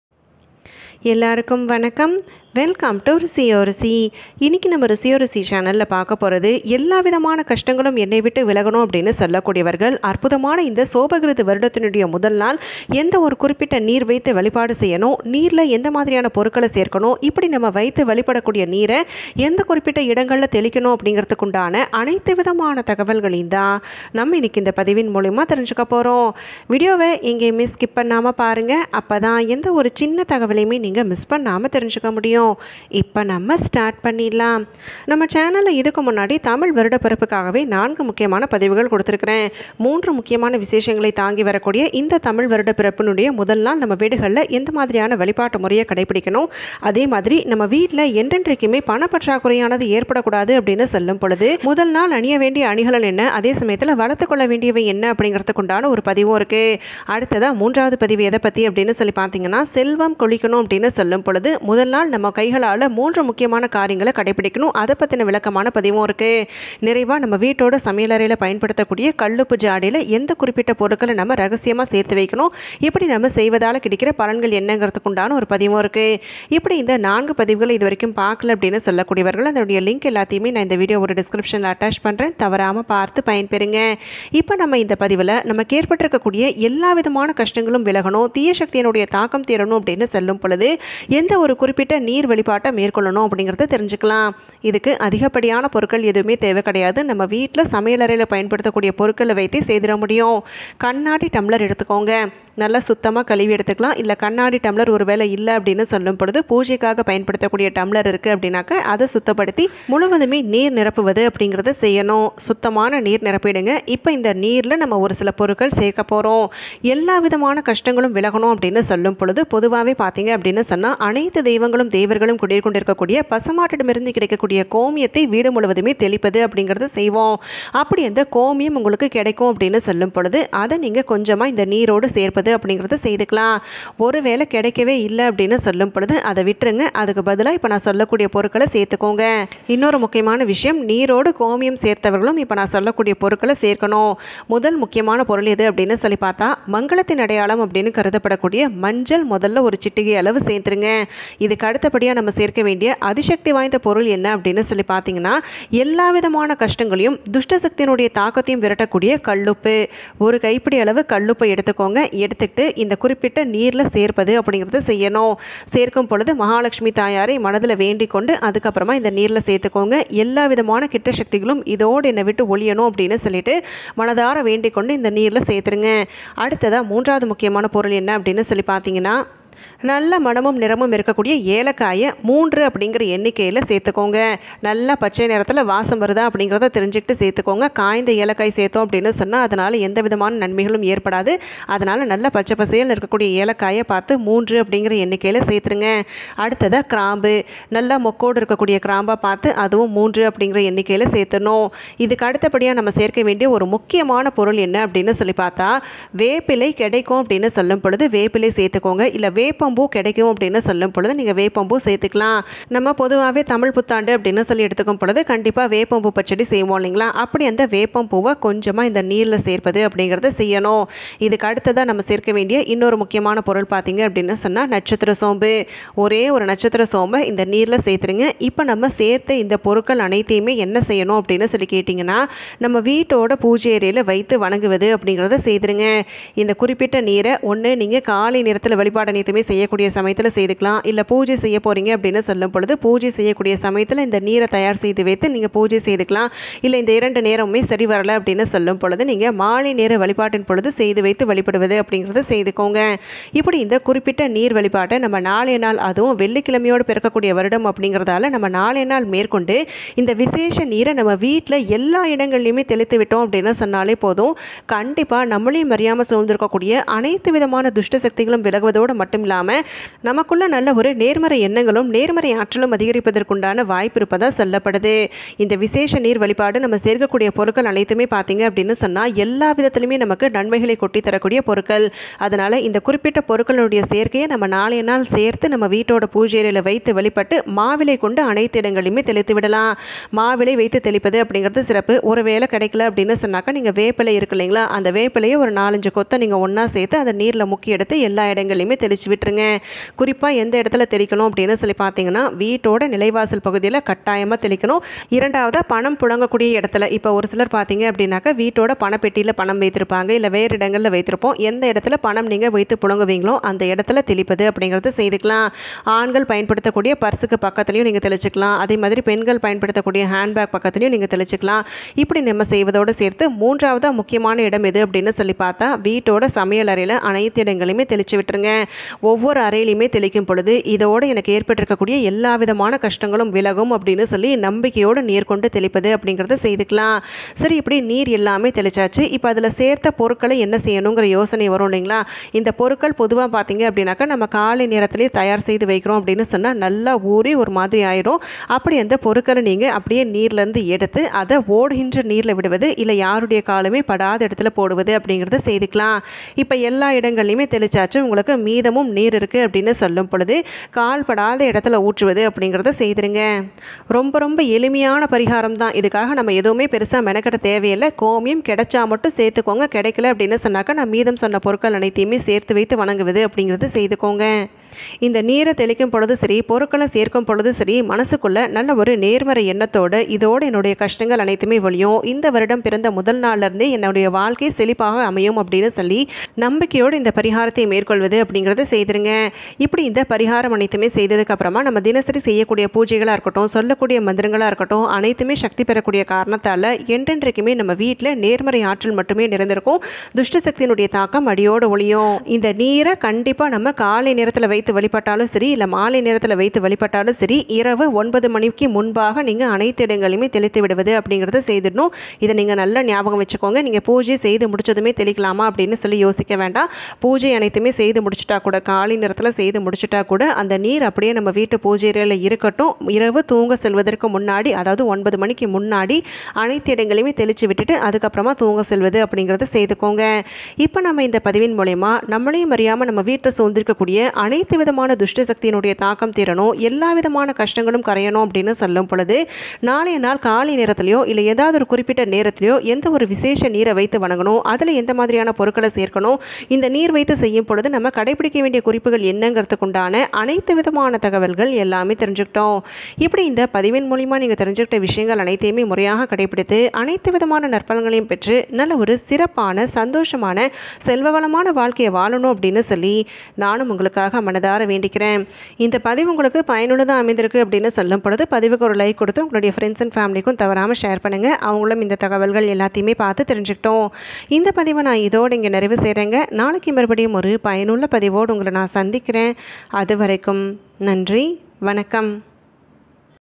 1 டம்ளர் நீர் போதும்|நாளை வெள்ளிக்கிழமையோடு வரும் வருடப்பிறப்பில் எல்லா பணக்கஷ்டமும் அடியோடு ஒழியும்
1 டம்ளர் நீர் போதும்.. வெள்ளிக்கிழமையோடு வரும் வருடப்பிறப்பில் எல்லா பணக்கஷ்டமும் அடியோடு ஒழியும்..

அவசர அவசிய பதிவு.. வருடப் பிறப்பு முதல் நாளில் சமையலறையில் உப்பு ஜாடியில் இதை சேருங்கள்.. பணம் மழை போல் குவியும்..: